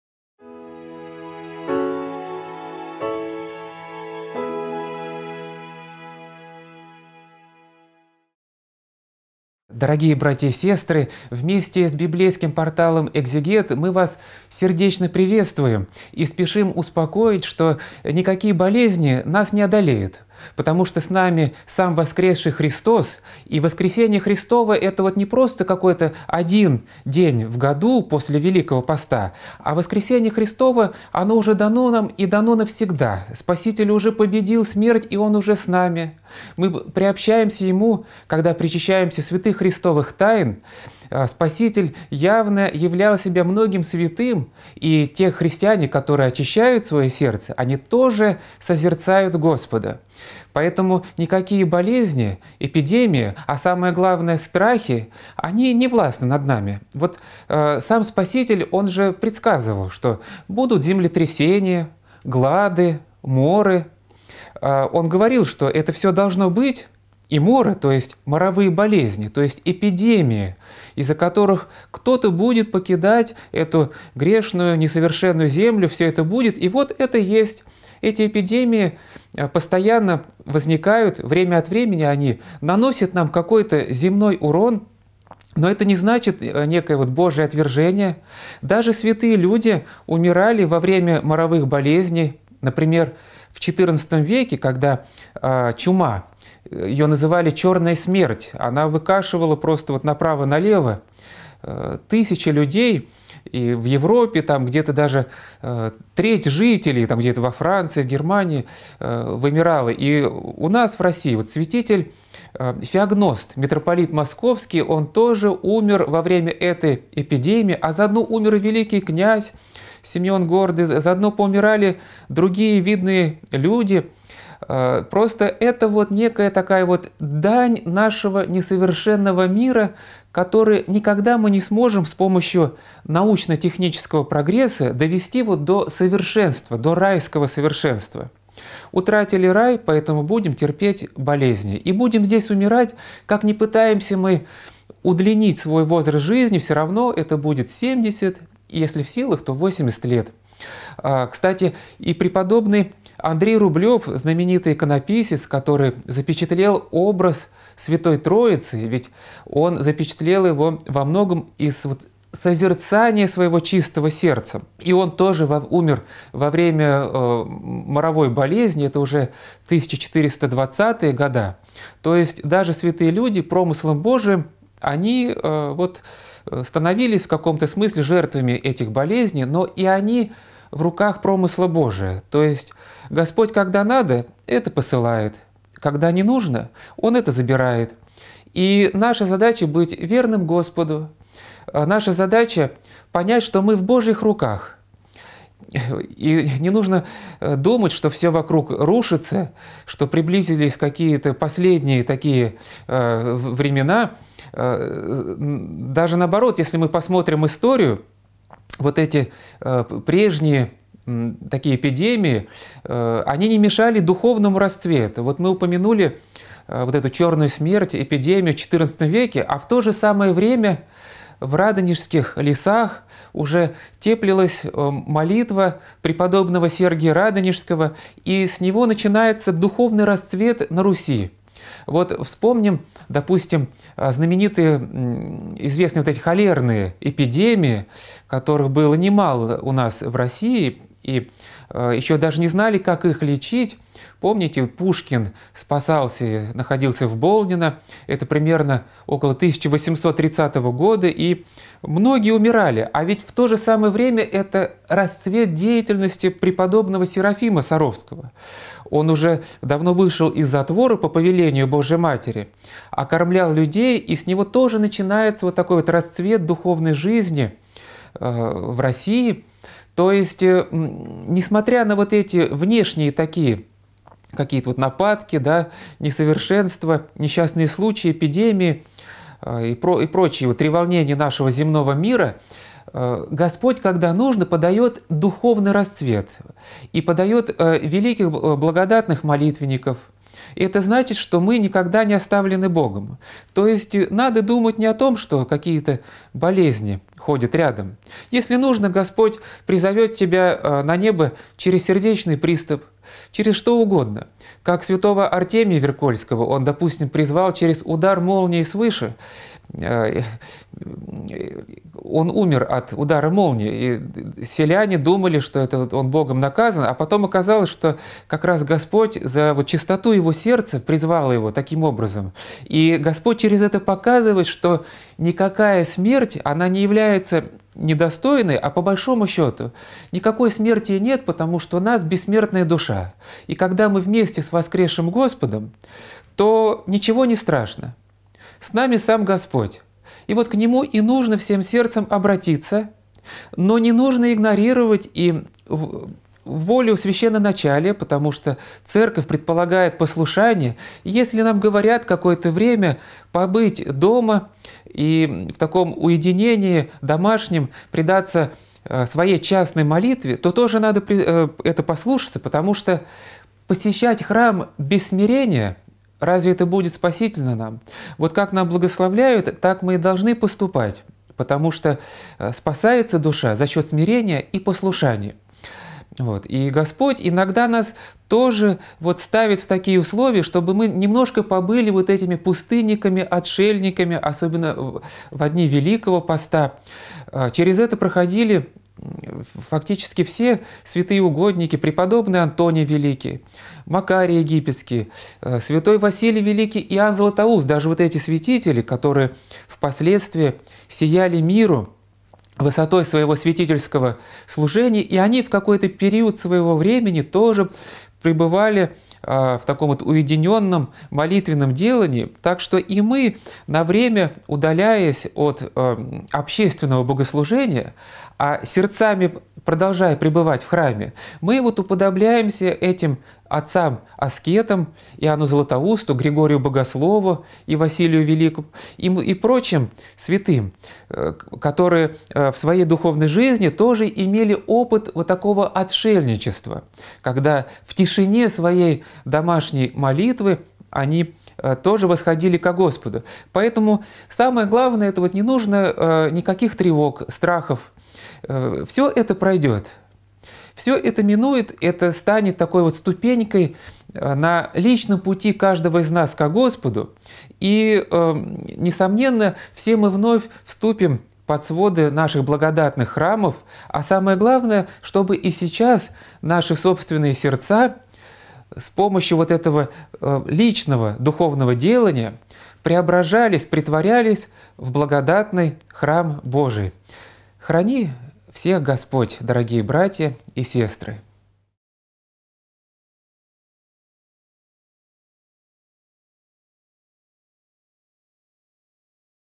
Эпидемии, страхи, коронавирус и Промысел Божий. Священник Валерий Духанин
В сегодняшнем чтении со священником Валерием Духаниным мы будем говорить об эпидемии, страхах, коронавирусе и Промысле Божьем и коснемся следующих тем:

🔸Присутствие Христа в нашей жизни.
🔸Страх не властен над верующими.
🔸Пророчества в Новом Завете.
🔸Эпидемии всегда были.
🔸Черная смерть.
🔸Смерть неизбежна.
🔸Промысел Божий.
🔸Бог обо всем ведает.
🔸Духовный рассвет на Руси.
🔸Наши молитвенники.
🔸Бессмертие души.
🔸Послушание и смирение.
🔸Опыт отшельников.
🔸Сохранение мира в душе.
🔸Духовный рост.
🔸Преображение души.